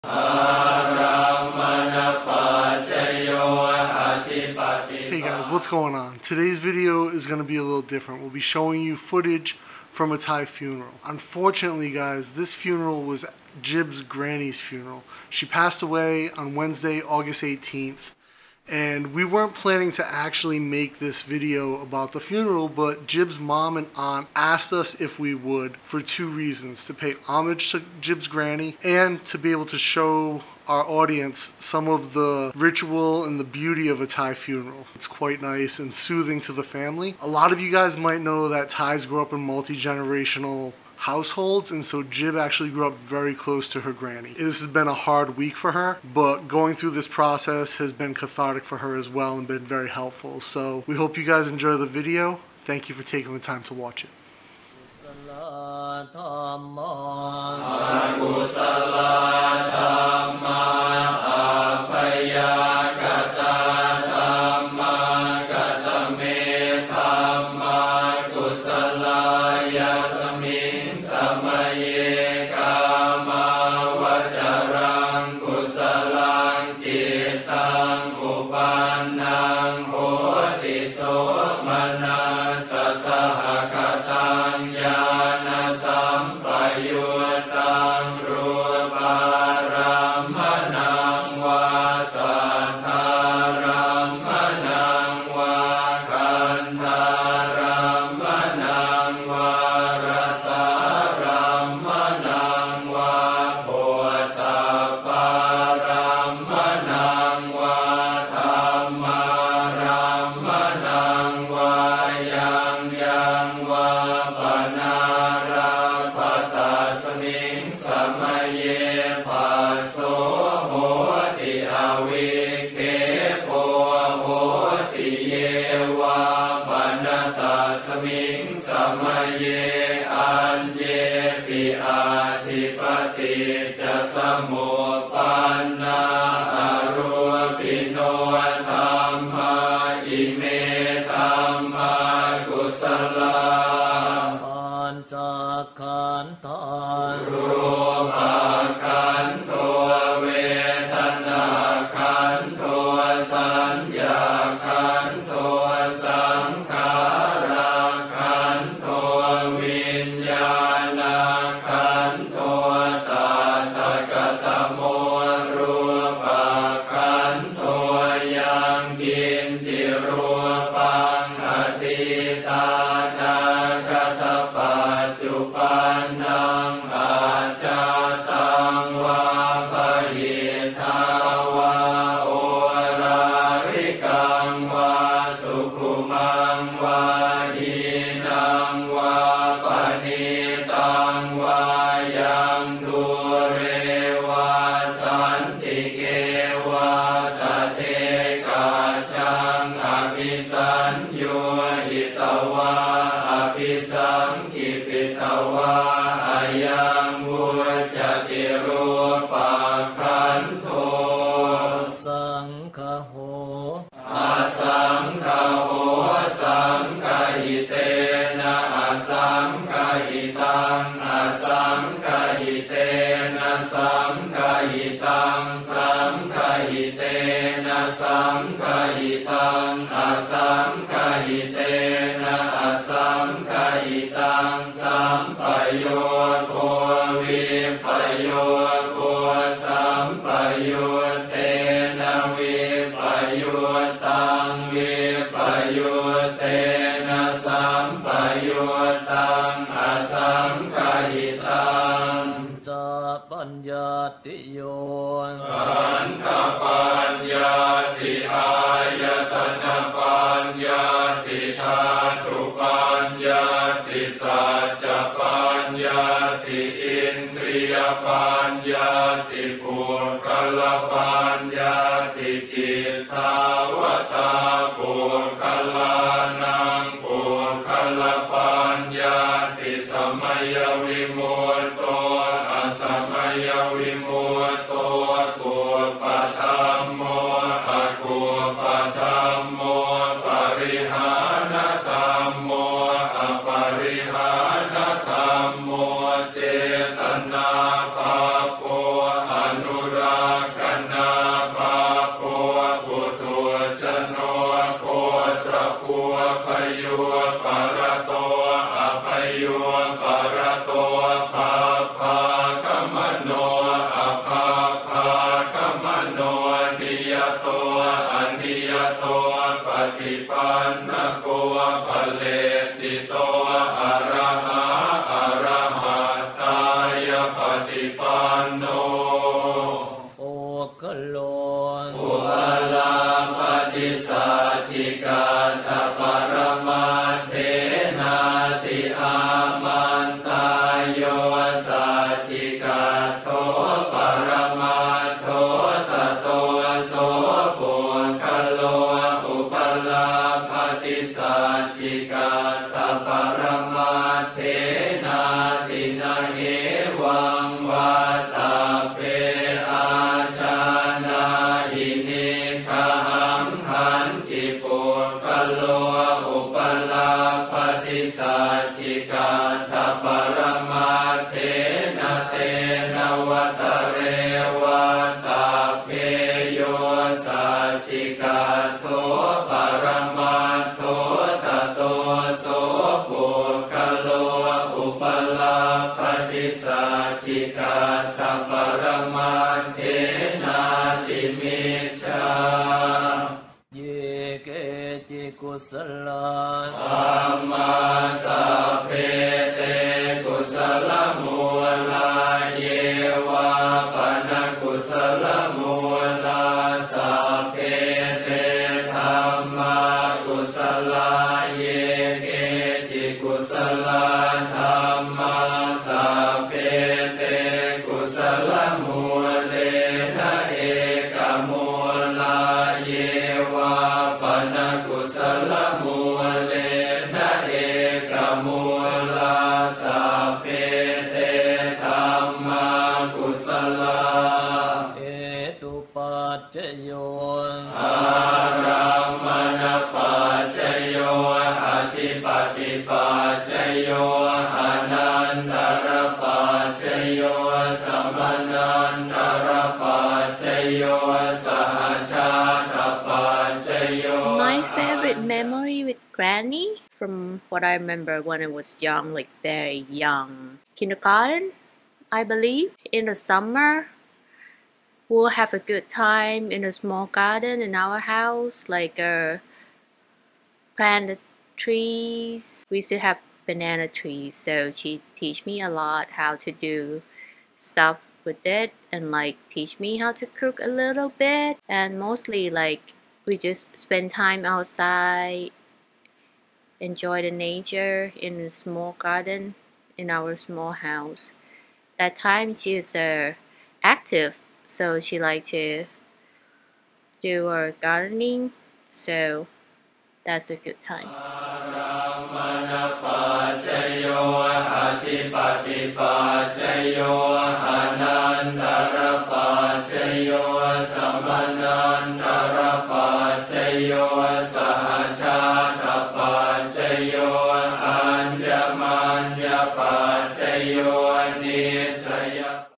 Thai Funeral and Cremation Ceremony in Bangkok
🔔 This video was shot in 5K on GoPro Hero 9 Black. Get Yours!

Let us know what you would like to see on our channel

This video shares the Thai Funeral and Cremation Ceremony at Wat Chao Am in Bangkok Thailand for Jibs Granny, Somkid​ Chewapricha. We have decided not to narrate the majority of the video and just leave the soothing Thai funeral chant.

Take action! Make your ideas a reality.

And of course, Stay Dapper!